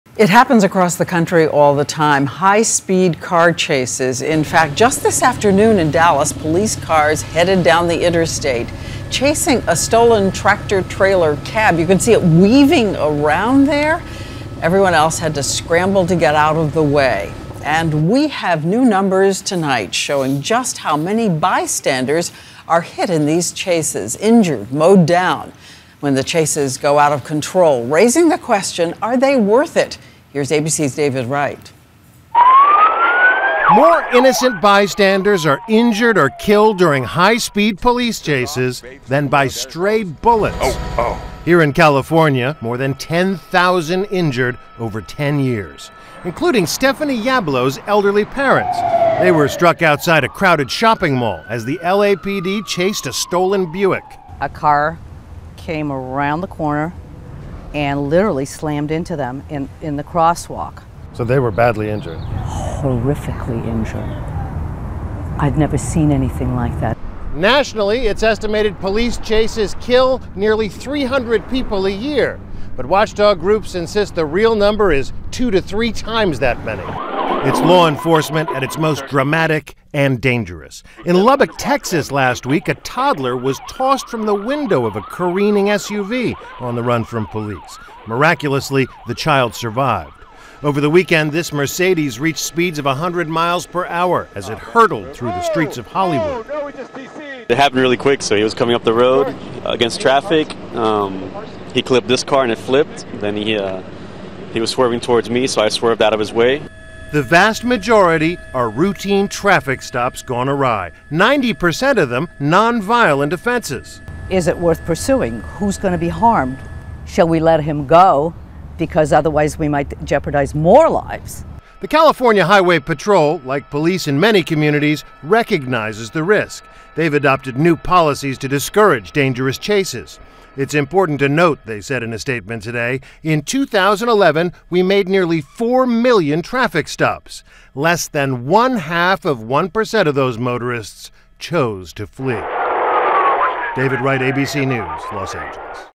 High-Speed Car Chases Caught on Tape: Cops Doing More Harm Than Good?
Police chases have caused thousands of injuries in CA in the past 10 years.